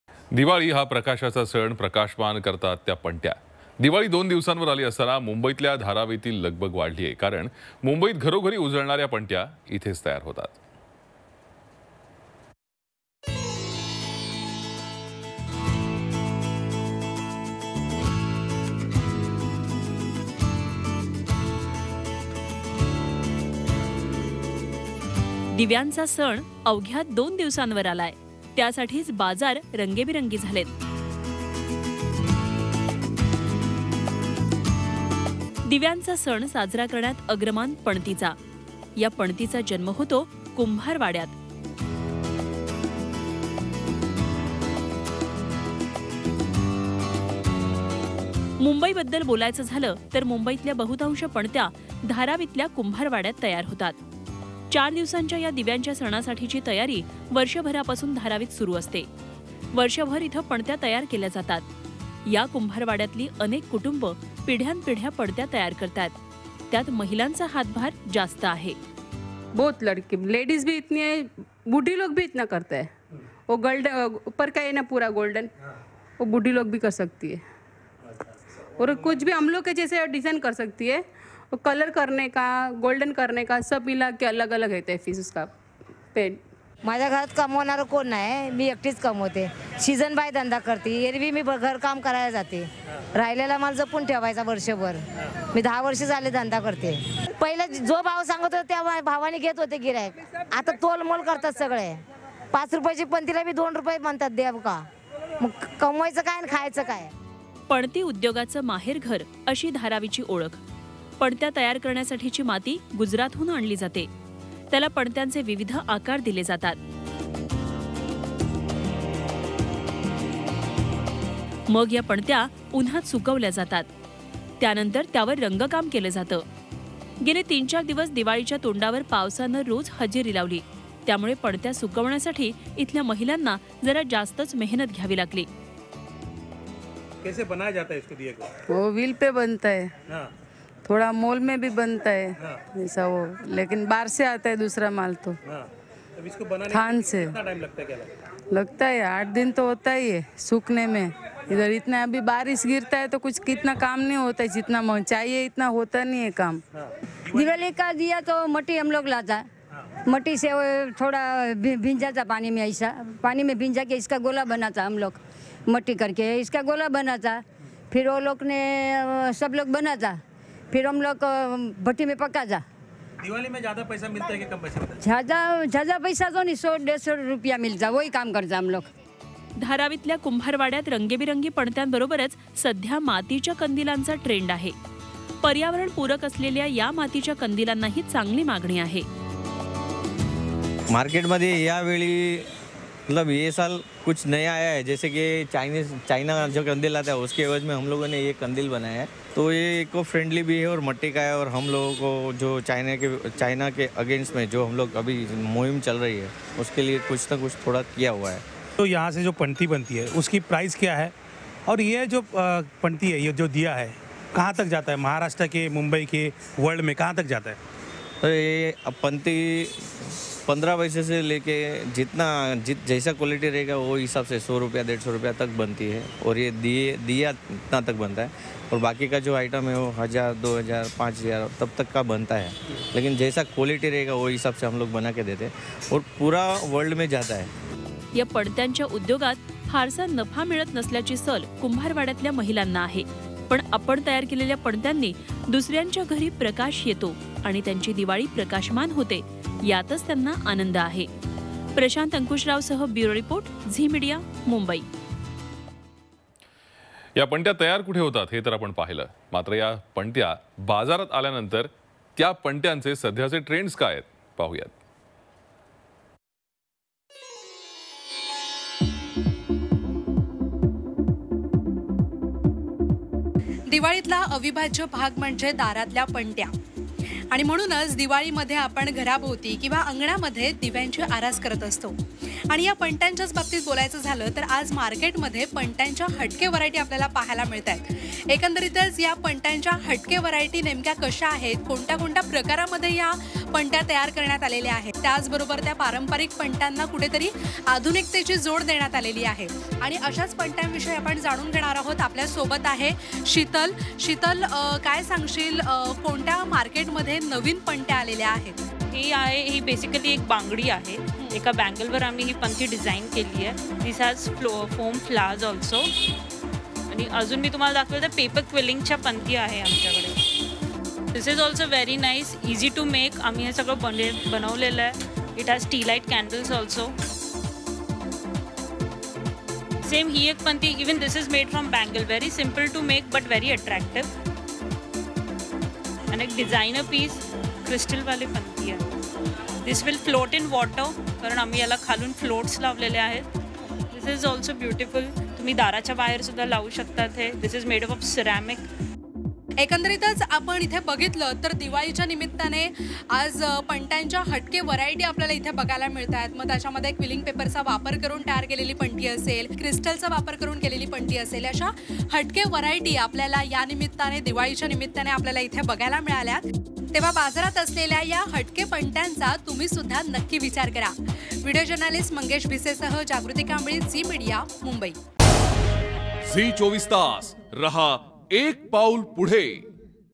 घरोघरी उजळणाऱ्या पणत्या कुंभारवाड्यात होतात तयार
Mumbai,Dharavi Diffrent Type Of Panti In Kumbharwada For Diwali And Trends Of Panti In Market